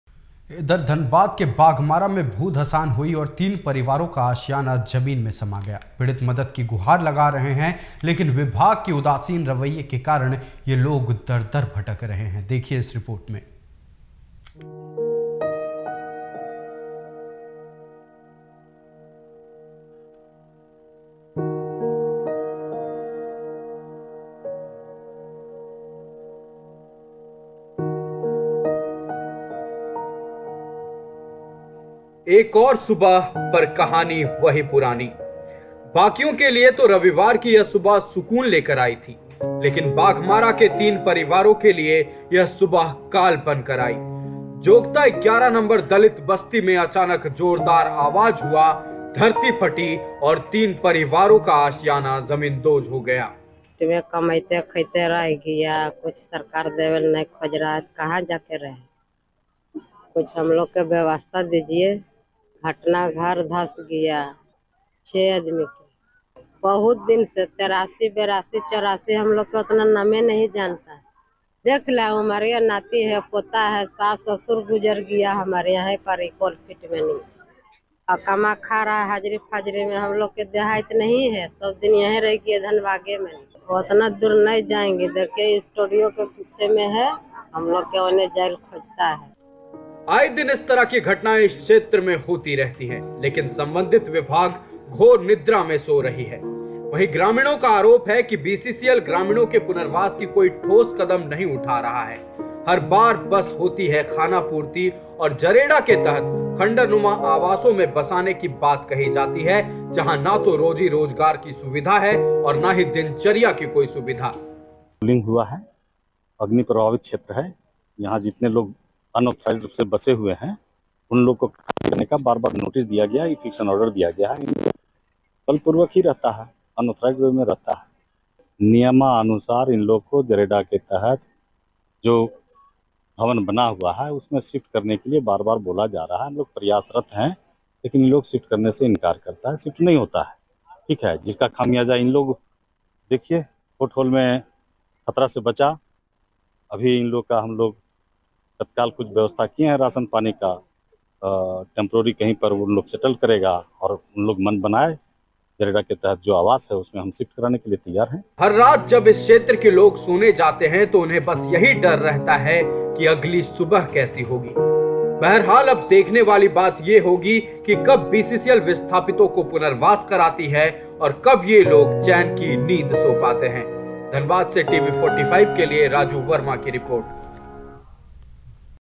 Dhanbad News : भू धंसान से छीना परिवारों का आशियाना, पीड़ित परिवार लगा रही मदद की गुहार
Dhanbad News: Families lost their homes due to landslide, victim's family pleading for help